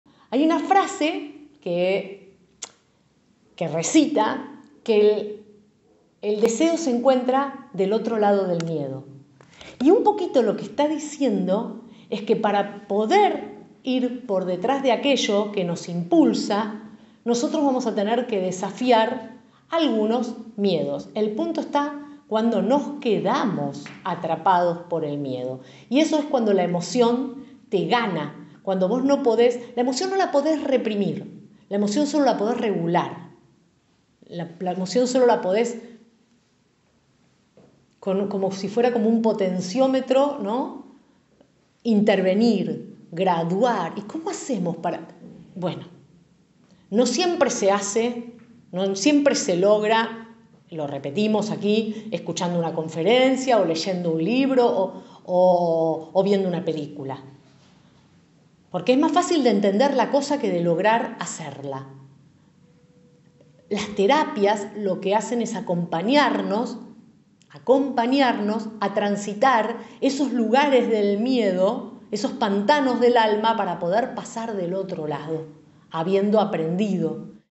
El deseo está del otro lado del miedo... ❤️🧠
¿Qué es la Inteligencia Emocional?
 “La capacidad de reconocer sentimientos propios y ajenos, de motivarnos y de manejar adecuadamente las relaciones”.  Daniel Goleman
Los invitamos a transitar la teoría  desde la perspectiva psicológica de la comunicación, aportes de las neurociencias y CON MUCHO HUMOR.
Aquí repasaremos  conceptos  que nos permitirán  aplicar las herramientas  necesarias  para reconocer y equilibrar las emociones, ser autores de nuestra historia y escribirla(nos) con lucidez.